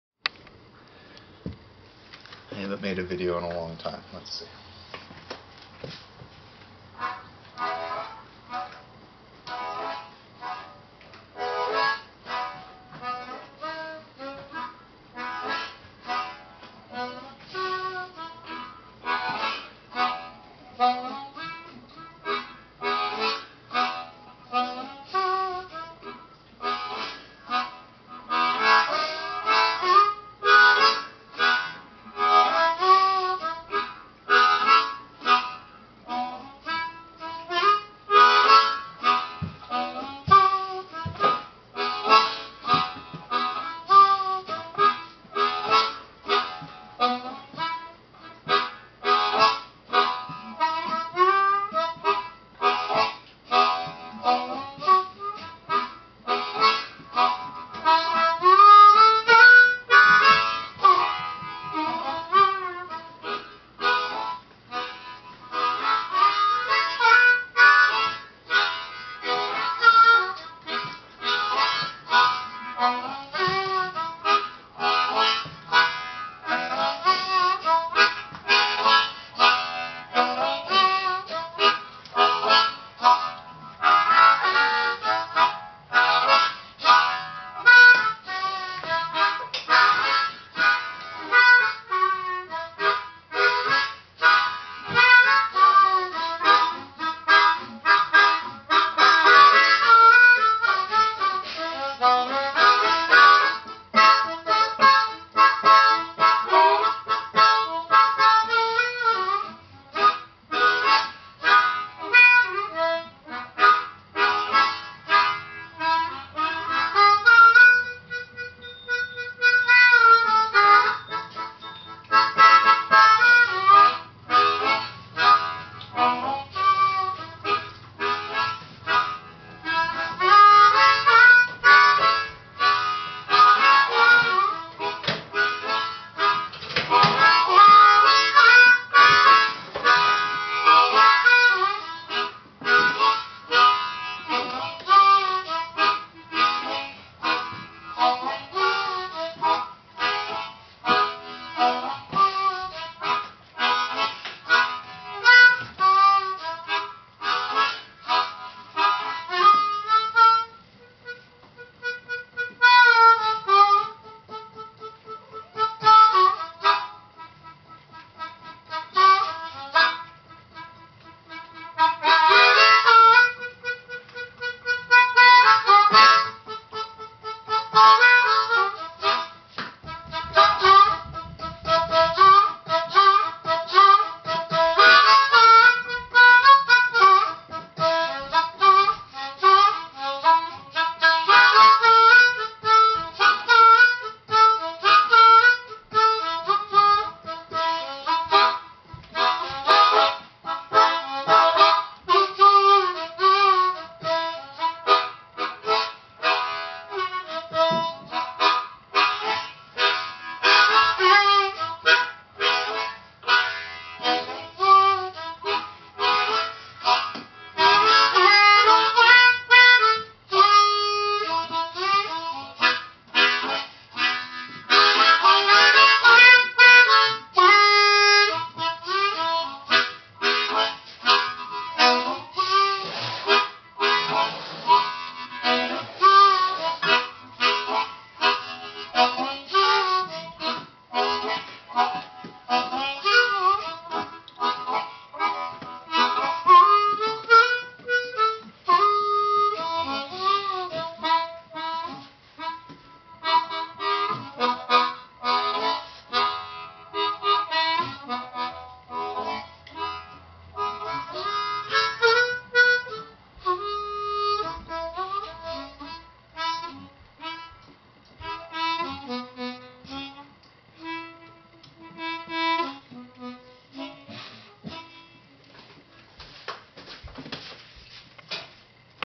A Birthday Cupcake Blow
I recorded this as Girlfriend pulled my special gourmet birthday cupcakes out of the oven!  Gourmet!